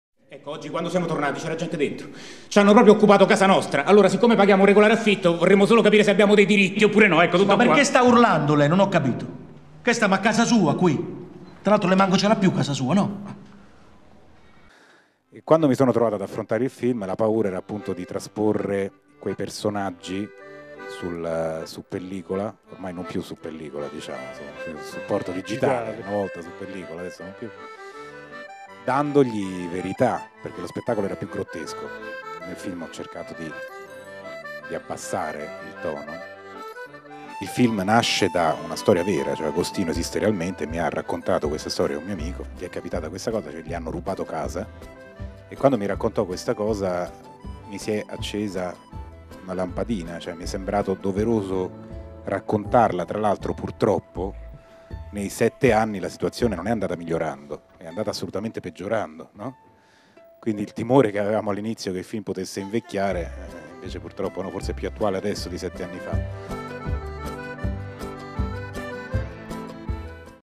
ROLANDO RAVELLO AL TUSCIA FILM FEST
Rolando Ravello al Tuscia Film Fest 2013 per "Tutti contro tutti". Viterbo, Arena Antennadsl - Complesso di San Carlo, 3 luglio 2013